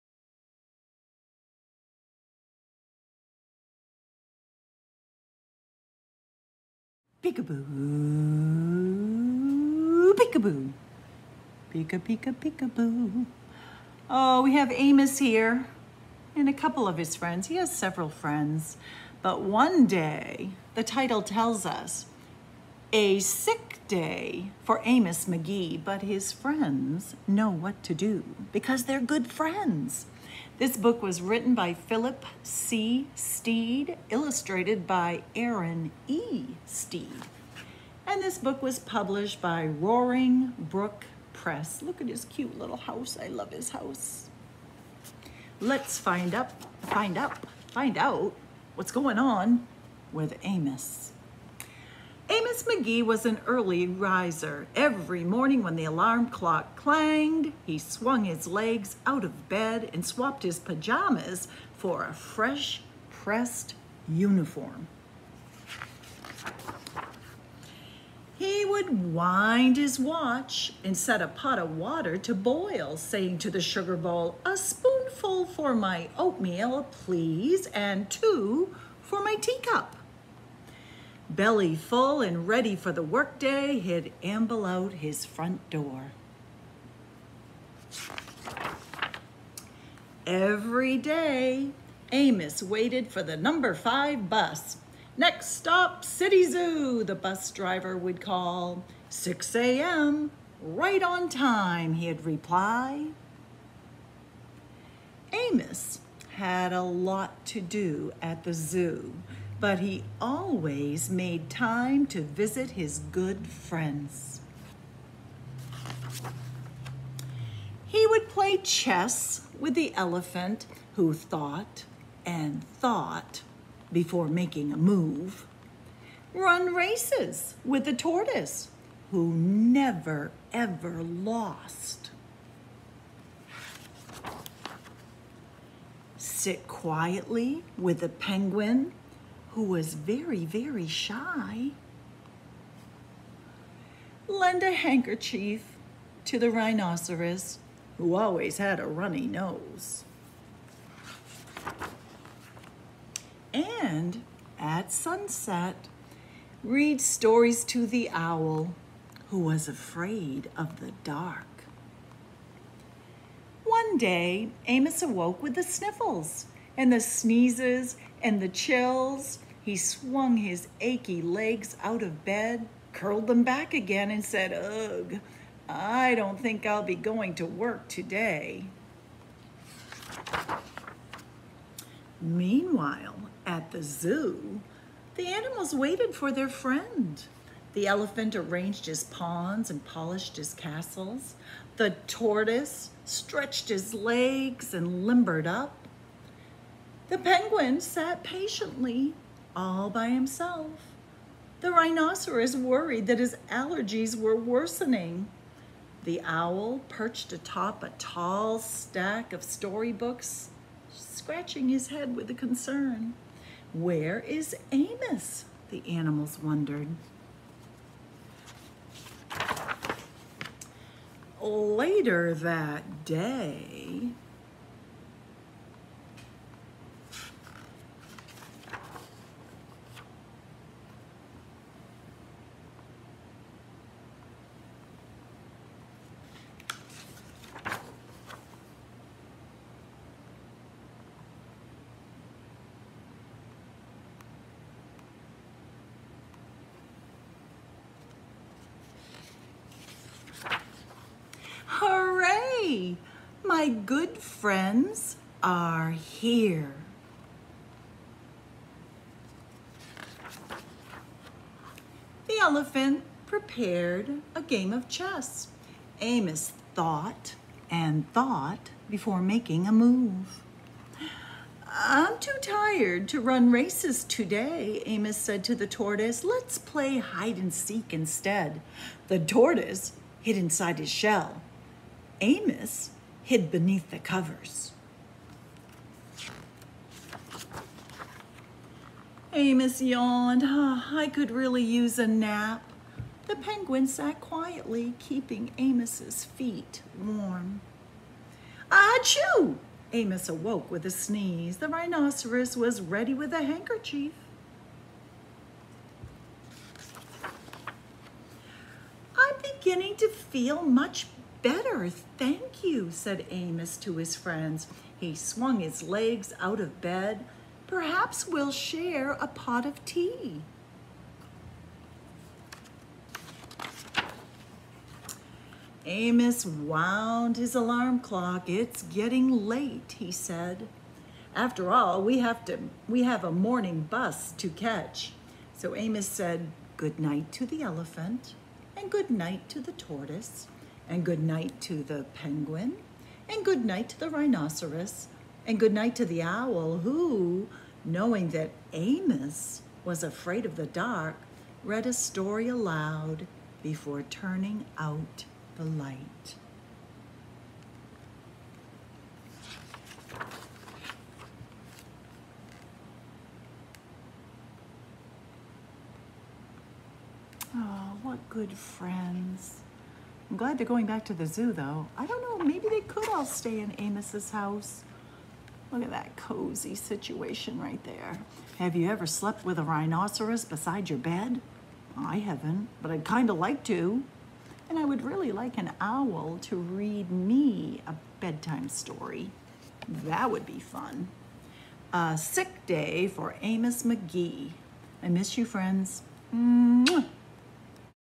Story Time with Miss Cindy
Read aloud picture book, A Sick Day for Amos McGee, written by Philip C. Stead and illustrations by Erin E. Stead. Published by Roaring Brook Press.